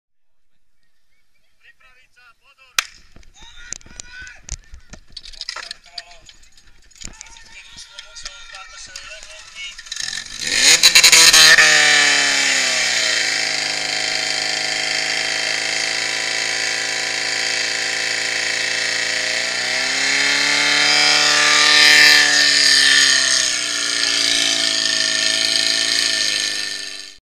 DHZ Bartošova Lehôtka - Kozárovce 2013 , Muži
DHZ Bartošova Lehôtka - Kozárovce , Muži 2013 , kamera na hlave
Výsledný čas: 16:80 hrubé hadice
1. miesto